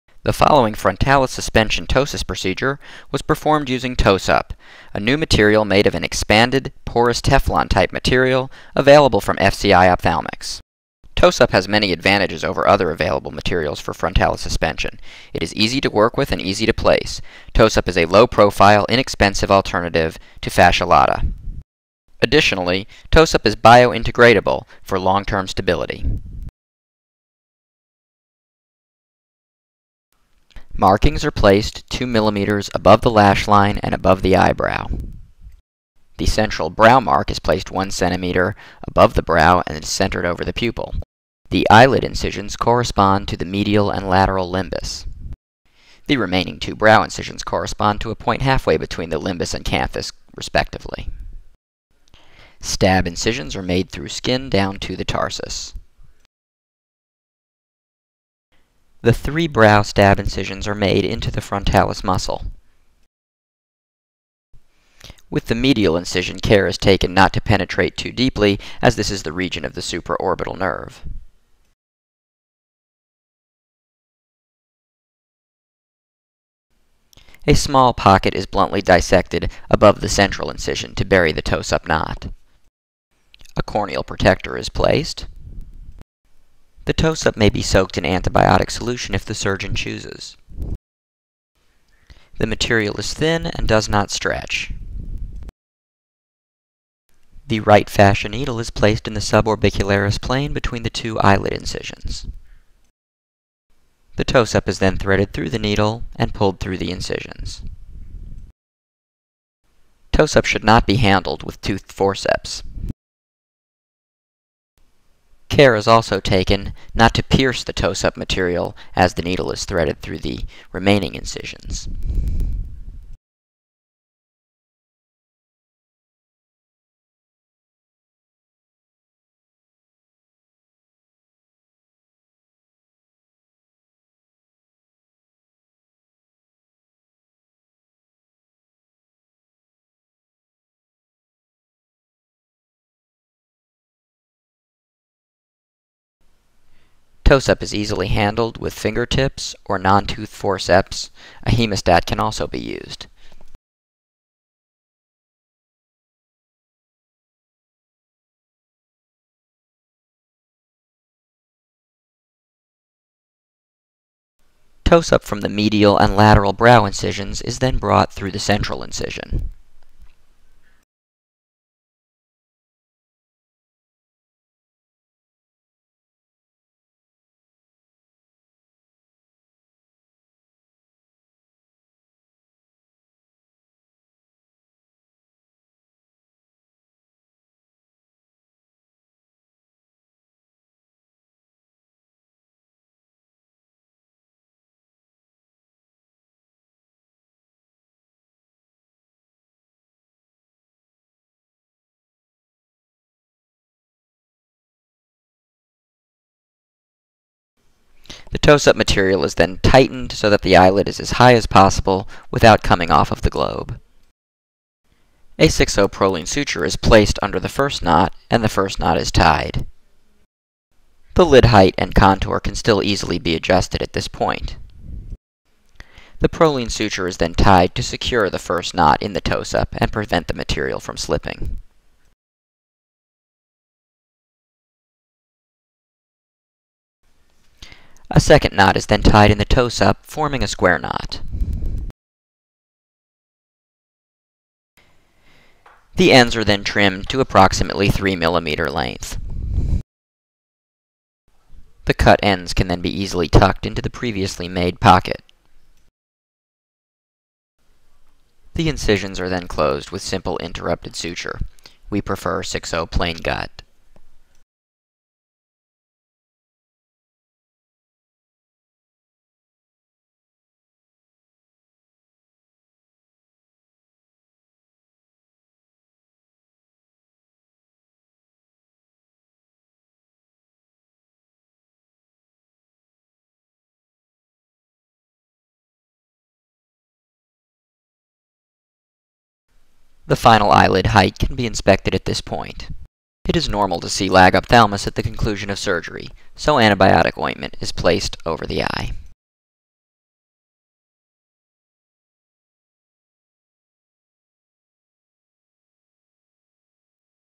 Ptose-Up® | ePTFE Frontalis Suspension | Features & Procedure | Exclusive | FCI Eyelids
FCI Ptose-up® is a strip of biomaterial made of ePTFE used to join the upper eyelid to the frontalis muscle in ptosis surgery with frontalis suspension: no need for autologous tissue.
Additionally, Ptose-up® is biocompatible, biointegrable and has an excellent tolerance. Its needles are with ultra sharp tips to facilitate the strip insertion